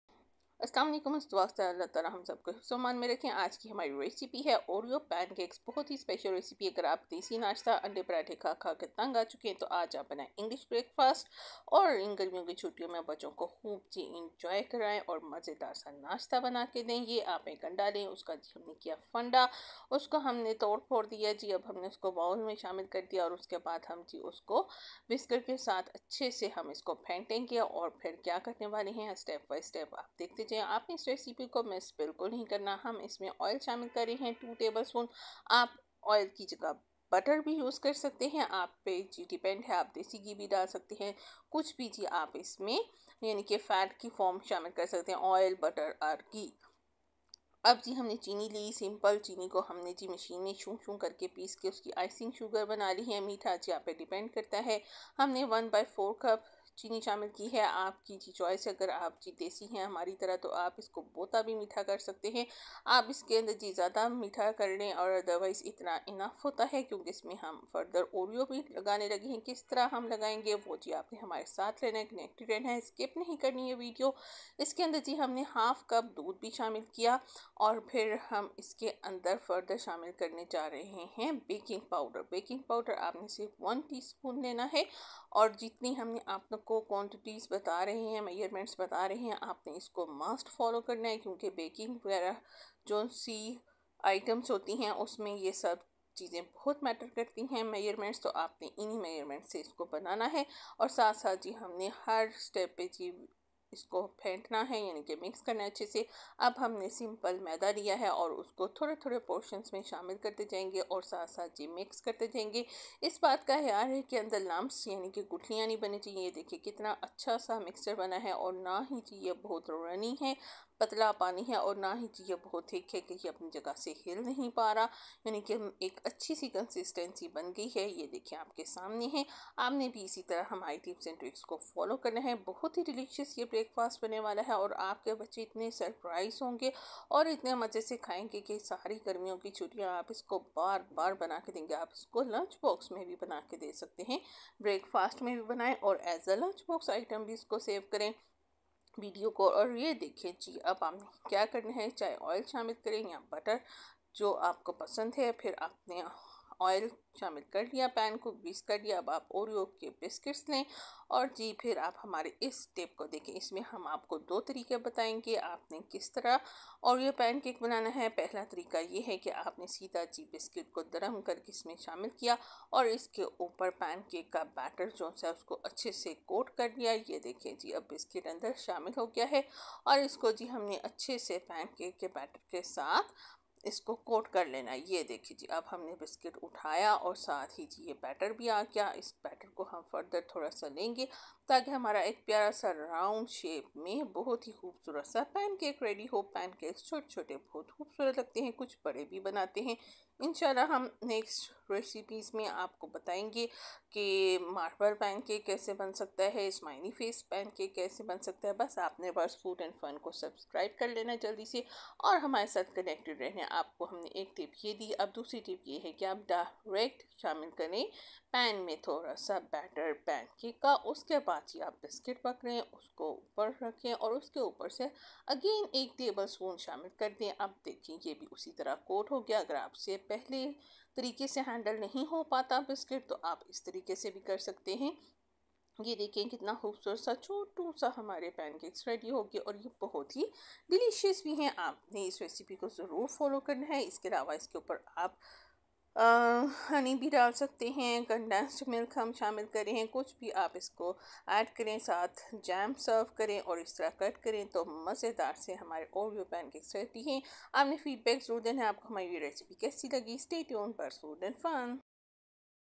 Delicious Oreo Pancakes Recipe | Easy and Quick Breakfast Idea
### Title

### Description
"Learn how to make mouthwatering Oreo pancakes with this easy and quick recipe! Perfect for breakfast or brunch, these fluffy pancakes are packed with Oreo goodness that kids and adults will love. Follow along as we show you step-by-step how to create this delicious treat.